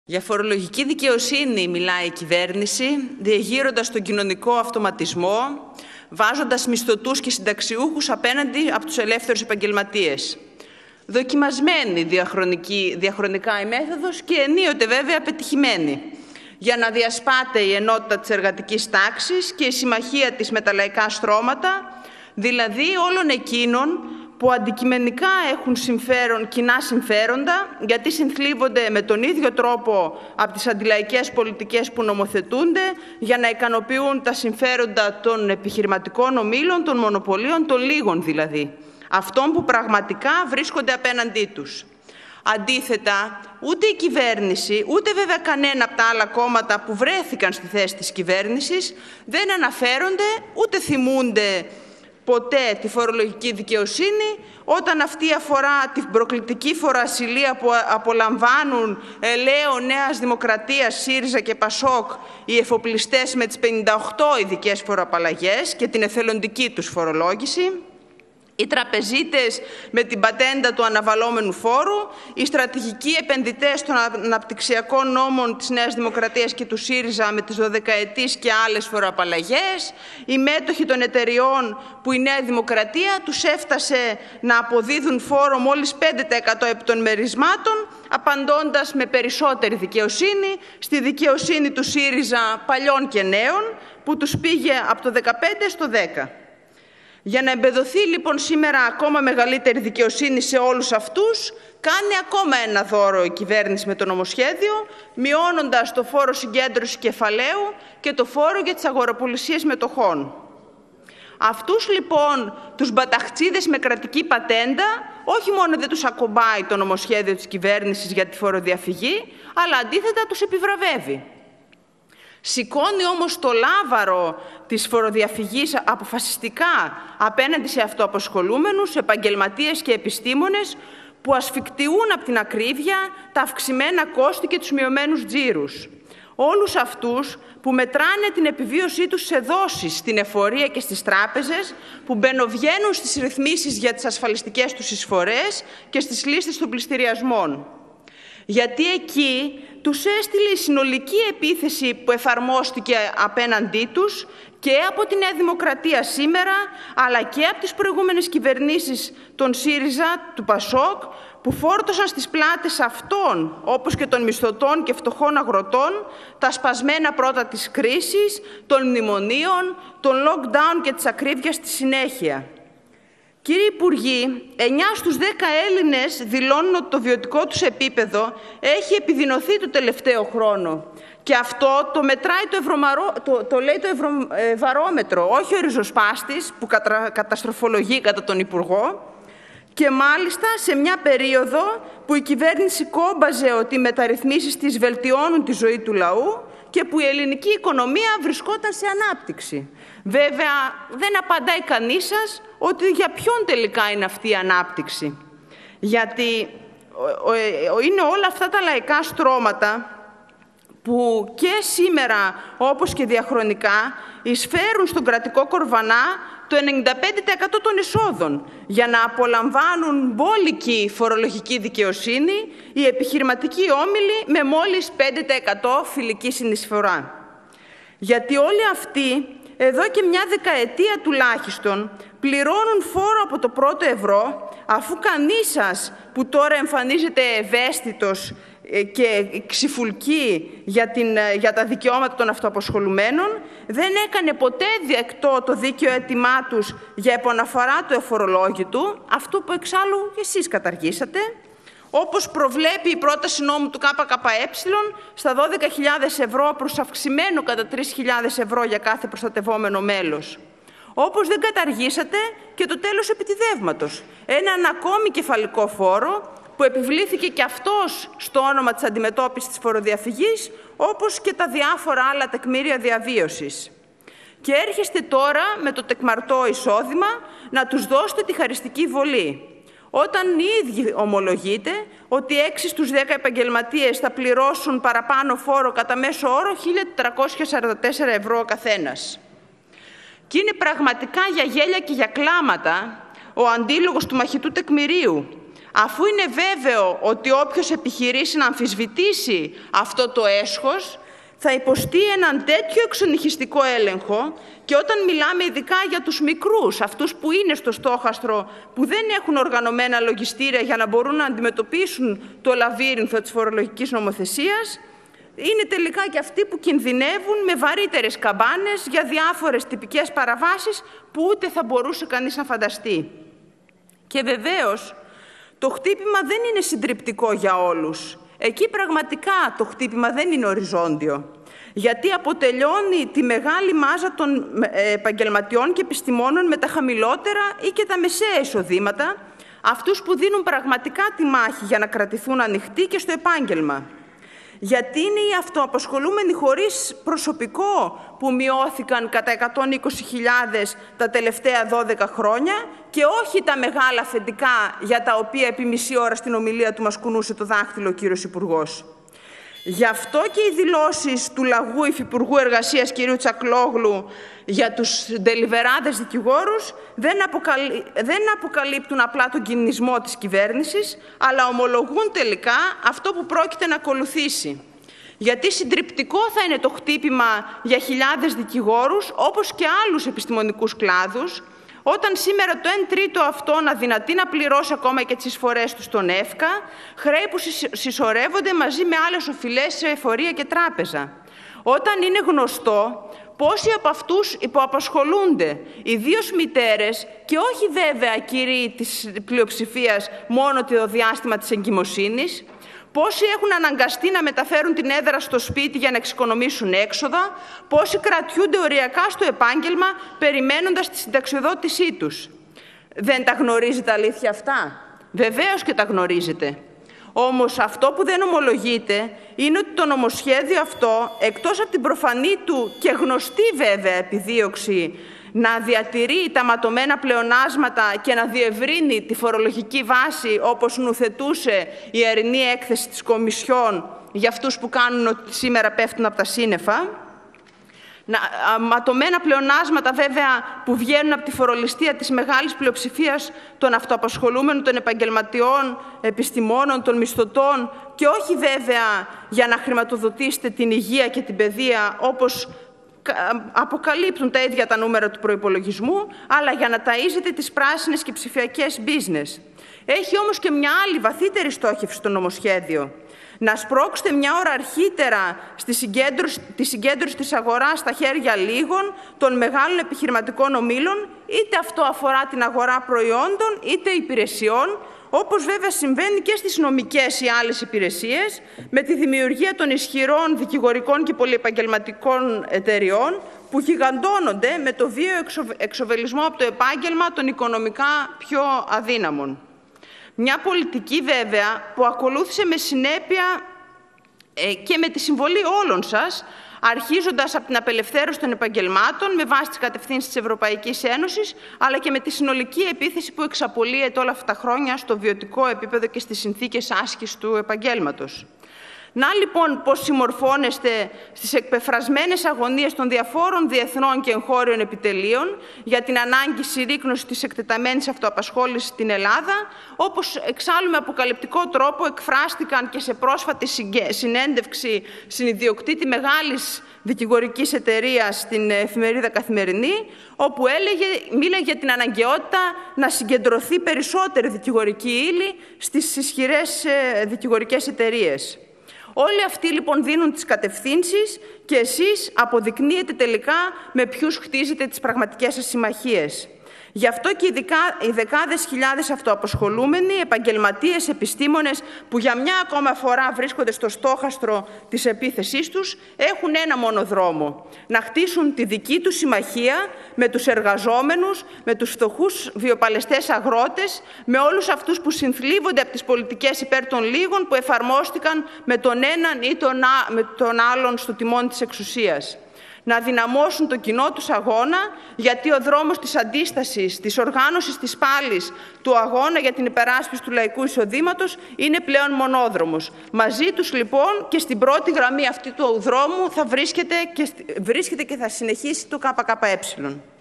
Μ. Κομνηνάκα: Βίαιος εξοβελισμός από το κάθε επάγγελμα των πιο αδύναμων
Το χτύπημα στο εισόδημα των αυτοαπασχολουμένων, επαγγελματιών, εμπόρων και επιστημόνων δεν είναι συντριπτικό για όλους, αποτελειώνει τη μεγάλη μάζα των εργαζομένων με τα χαμηλότερα ή και μεσαία εισοδήματα, επισήμανε στην ομιλία της η βουλευτής του ΚΚΕ Μαρία Κομνηνάκα, μιλώντας στην Ολομέλεια επί του φορολογικού νομοσχεδίου.

Κατήγγειλε την κυβέρνηση που χτυπά τους μικρούς και αφήνει στο απυρόβλητο το μεγάλο κεφάλαιο τονίζοντας ότι εκτός από τους διακηρυγμένους στόχους του νομοσχεδίου για διεύρυνση της φορολογικής βάσης και τα ματωμένα πλεονάσματα, το νομοσχέδιο έχει μία βαθύτερη στόχευση, τη συγκέντρωση της αγοράς στα χέρια λίγων, είτε αφορά αγορά προϊόντων, είτε αφορά αγορά υπηρεσιών, με τον βίαιο εξοβελισμό από το κάθε επάγγελμα των πιο αδύναμων.

Επικαλούμενη  στοιχεία του Ευρωβαρόμετρου, σημείωσε πως 9 στους 10 εργαζόμενους δηλώνουν επιδείνωση του βιοτικού επιπέδου και σε μια περίοδο, μάλιστα, που η κυβέρνηση κόμπαζε για την «ανάπτυξη» της ελληνικής οικονομίας, που δεν αφορά άλλους από τους επιχειρηματικούς ομίλους και όχι βέβαια τον λαό και τα λαϊκά στρώματα. Κατήγγειλε και την υποκρισία των άλλων αστικών κομμάτων που κυβέρνησαν, που τώρα εμφανίζονται ότι στηρίζουν τους ΕΒΕ, όταν επέβαλαν και εφάρμοσαν τον φόρο από το πρώτο ευρώ, ενώ ποτέ καμία κυβέρνηση δεν δέχθηκε την επαναφορά του αφορολόγητου, που οι ίδιοι κατάργησαν.